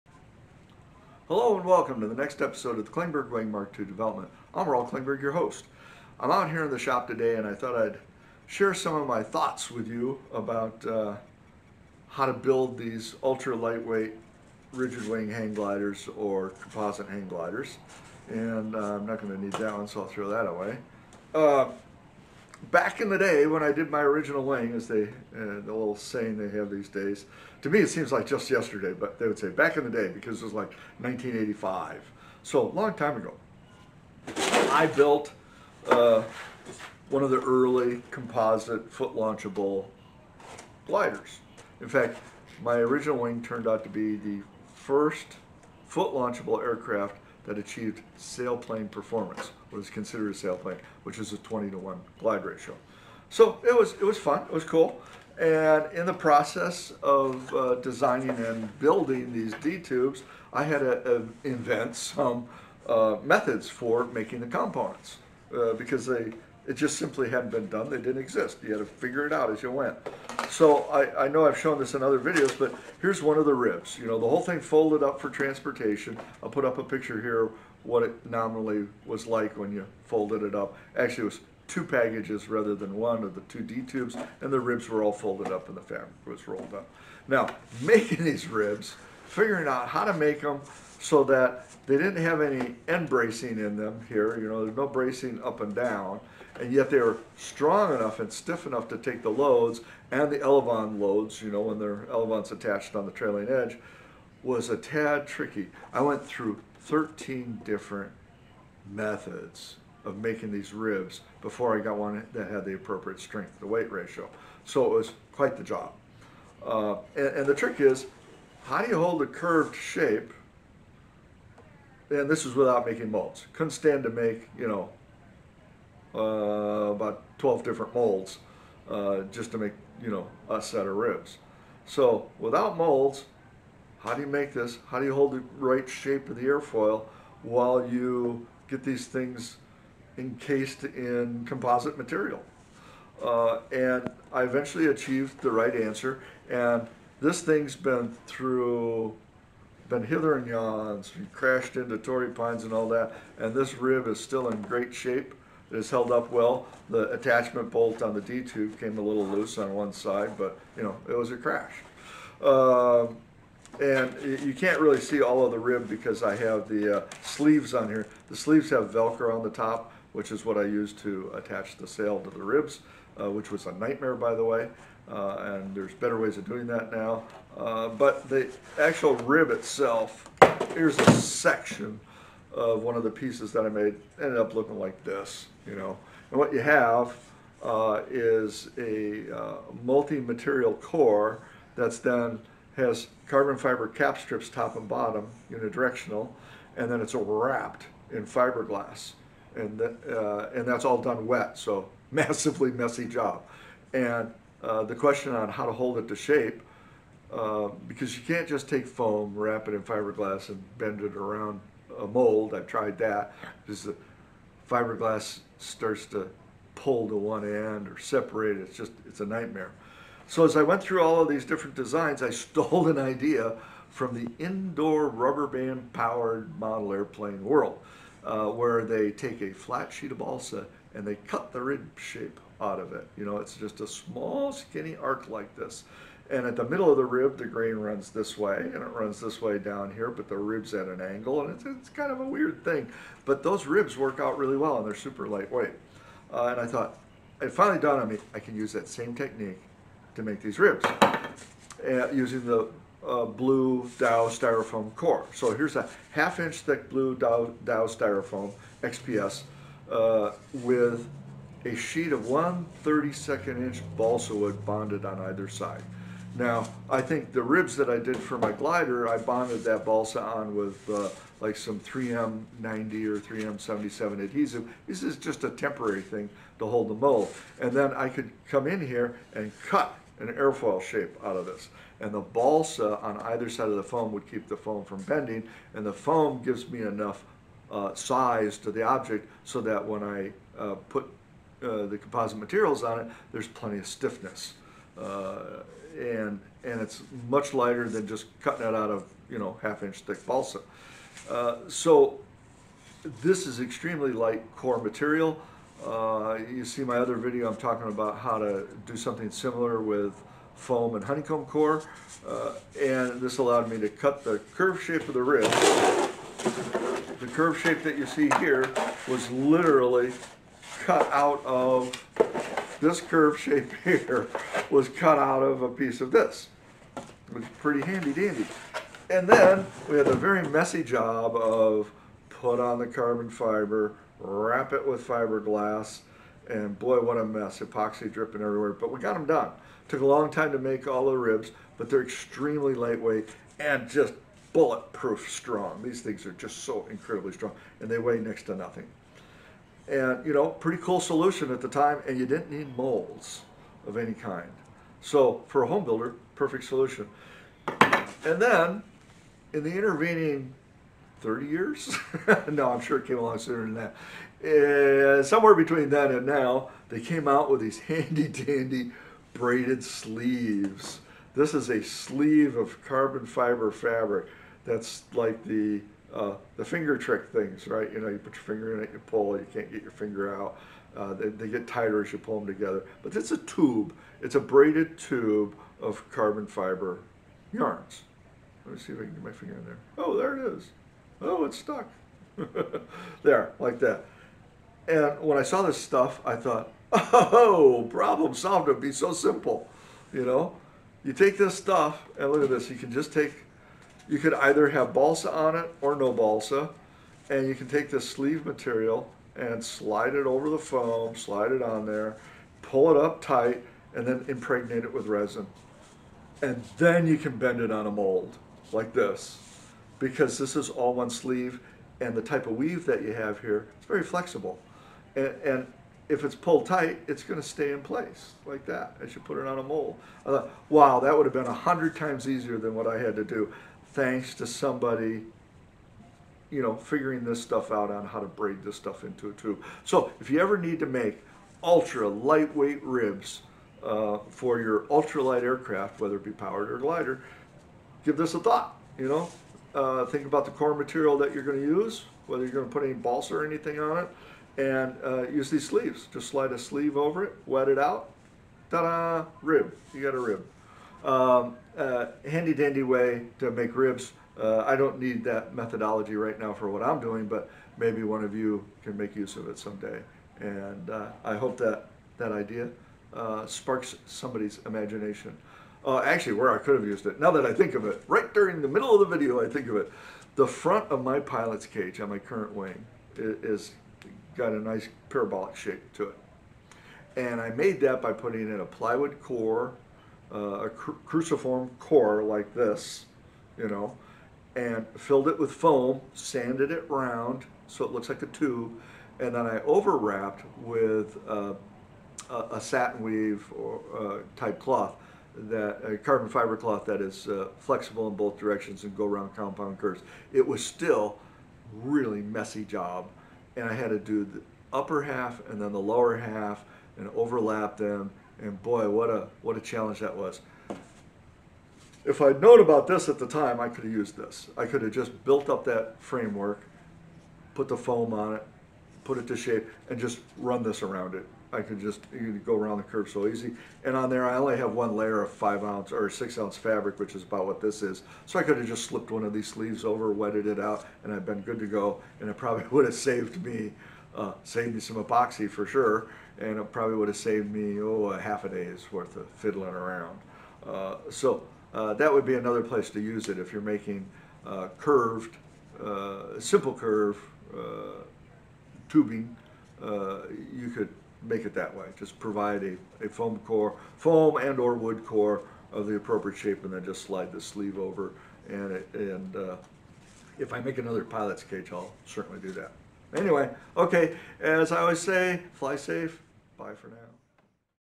Making Compound Curved Composite Shapes - Klingberg Wing MkII Development
This channel presents key activities related to my development of a new, 15m span, foot-launchable sailplane.